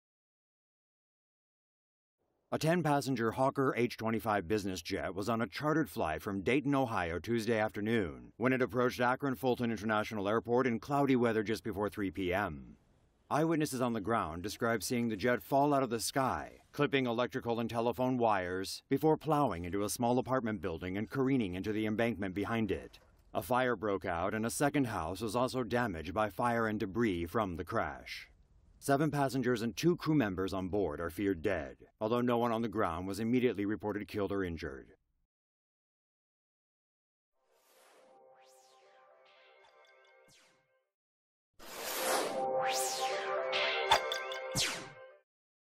Ohio plane crash: Nine feared dead as jet crashes into small apartment building
Originally published on 11 November, 2015

Authorities are investigating the cause of a small plane crash that took place in a residential neighborhood in Ohio on Tuesday that left no survivors.

The Akron Beacon Journal reports that a 10-passenger Hawker H25 business jet was on a chartered flight from Dayton, Ohio Tuesday afternoon.

The small aircraft was approaching Akron Fulton International Airport in cloudy weather just before 3 p.m, when eyewitnesses on the ground described seeing the jet fall out of the sky, clipping electrical and telephone wires before plowing into a small apartment building and careening into the embankment behind it. Both the jet and the apartment building went up in flames as a second house was damaged by fire and debris from the crash, according to WKYC.

The owner and operator of the plane confirmed that seven passengers and two crew members were on board, reported WOIO. Officials have said there were no survivors, and that there seemed to be no casualties on the ground, but would not confirm how many people are dead.

Power lines brought down by the crash also caused some 1,500 customers in Akron to lose power following the incident.